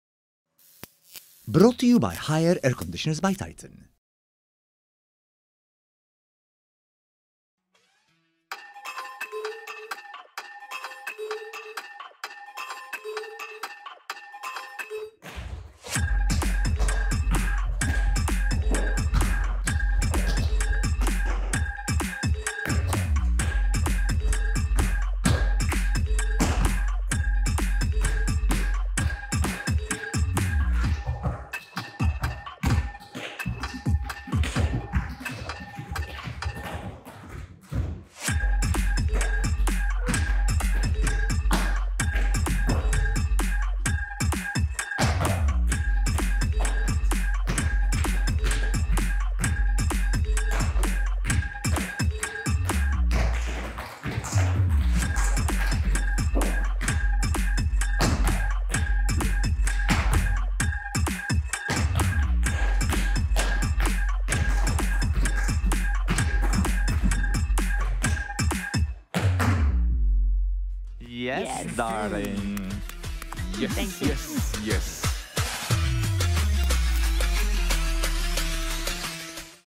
Kim Ellul- Brillanti Dance- Bravura Challenge
With just two hours to choreograph and perform a piece to a randomly selected track by the judges, Kim Ellul aces the Bravura Challenge!